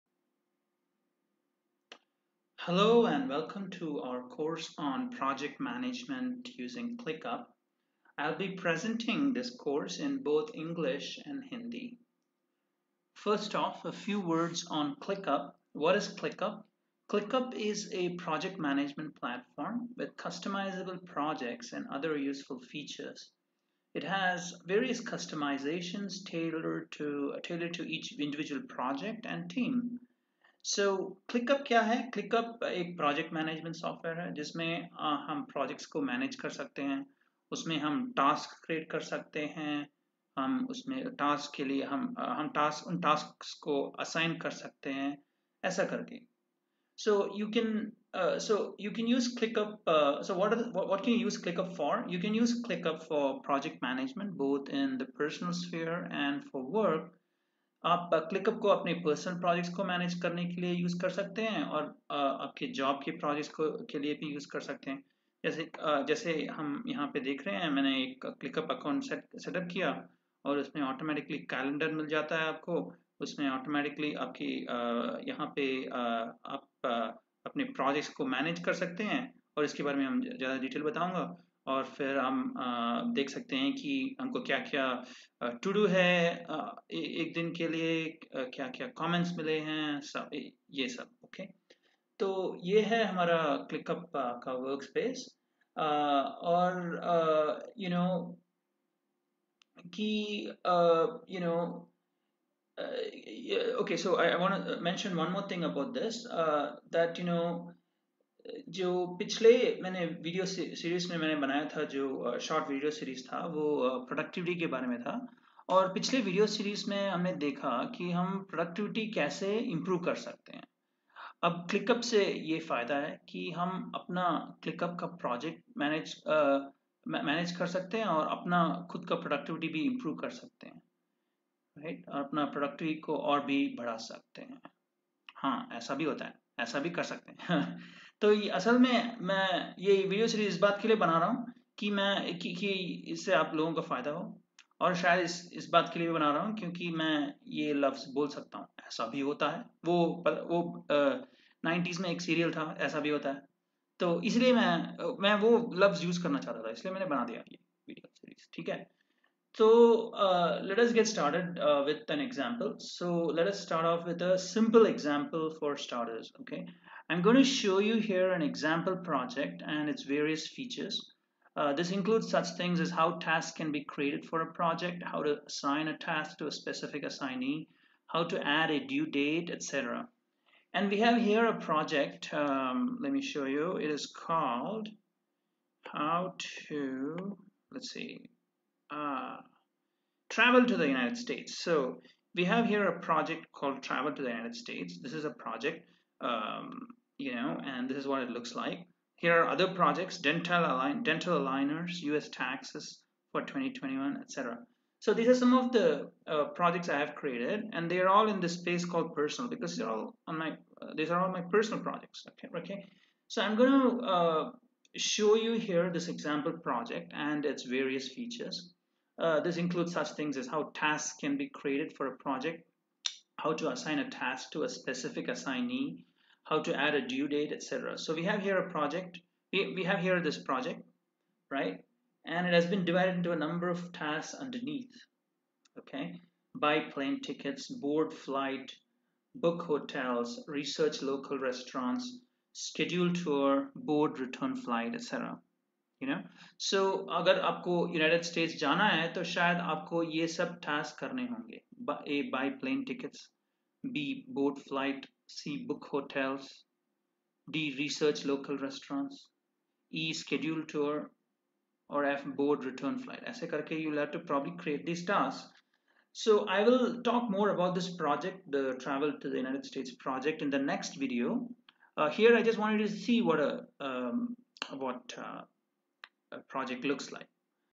Want to Learn Programming? Learn both Project Mgt and Programming! Learn Project Mgt using ClickUp
Want to learn programming? If so. read on!

Interested in learning programming? We have just the thing for you!

Learn about our new programming classes, offered in association with the Esperanto and ConLang Society of South India:

You just can't go wrong with ESSh!

This is the first video in the set of 3 introductory videos for the course "Project Management using ClickUp".

"क्लिकअप का उपयोग: Project Management using ClickUp" पाठ्यक्रम के लिए यह पहला वीडियो है।

This mini-course was originally created because we couldn't find good enough video tutorials that were short enough and comprehensive enough for speakers of Hindi.

We needed employees to learn ClickUp as part of the onboarding process. So, we prepared this video series. We are now making this video available for free to the public as part of Corporate CSR.